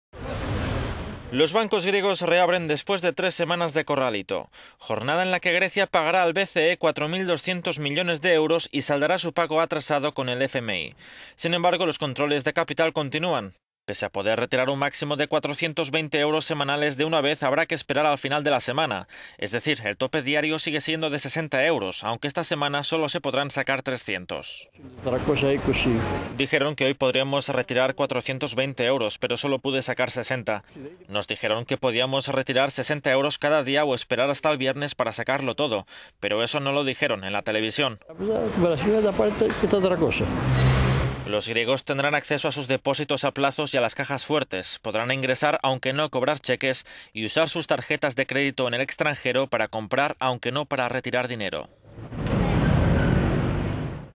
Reabren los bancos en Grecia, aunque se mantienen los controles de capital
Los bancos griegos reabren después de tres semanas de corralito. Jornada en la que Grecia pagará al BCE 4.200 millones y saldará su pago atrasado con el FMI.

Sin embargo, los controles de capital continúan. Pese a poder retirar un máximo de 420 euros semanales de una vez, habrá que esperar al final de la semana; es decir, el tope díario sigue siendo de 60 euros, aunque esta semana solo se podrán sacar 300.

"Dijeron que hoy podríamos retirar 420 euros, pero solo pude sacar 60. Nos dijeron que p…